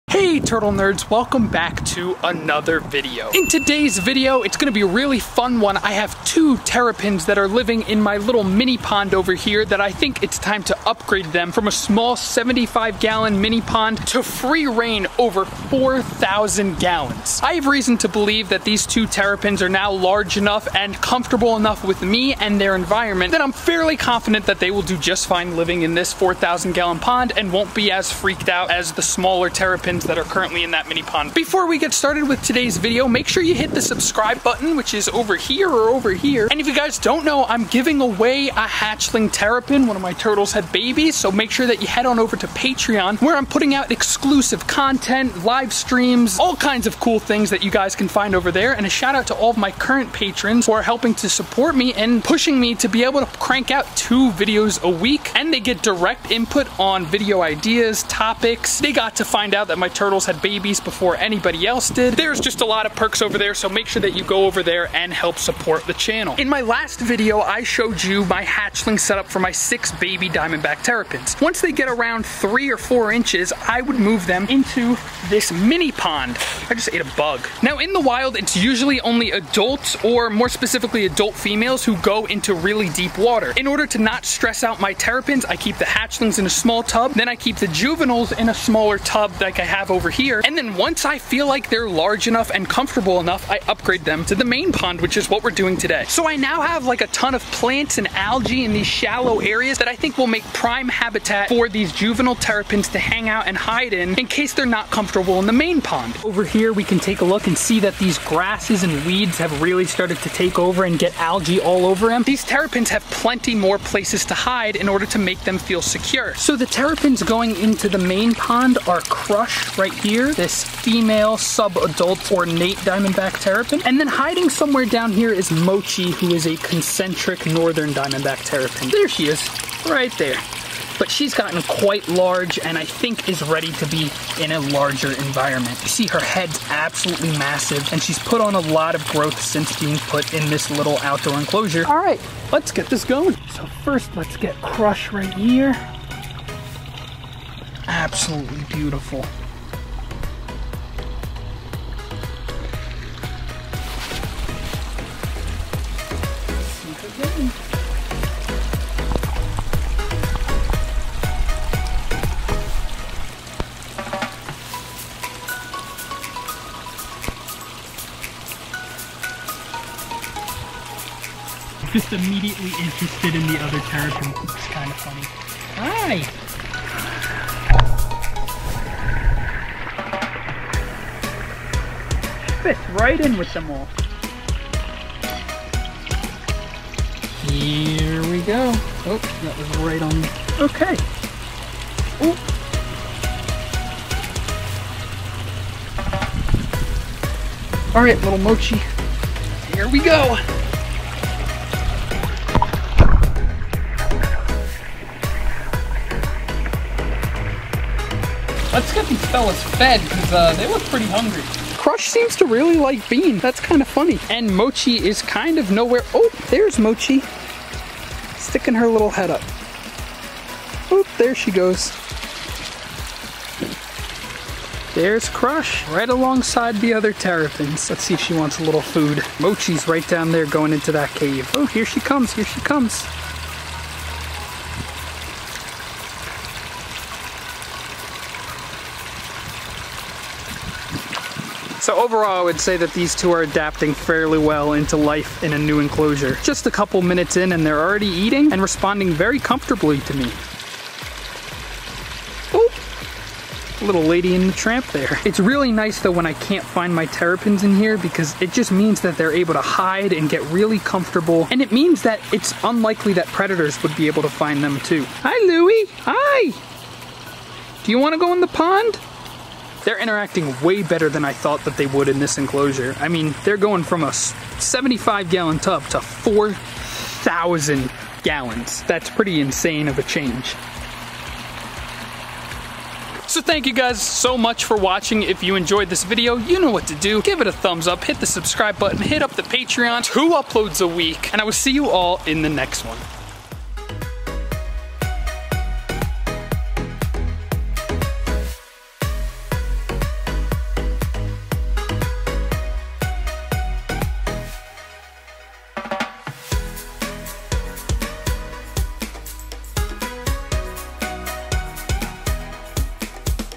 Releasing TURTLES Into My POND!
Hey Turtle Nerds!  Today I release my juvenile terrapins from the mini pond to the main pond!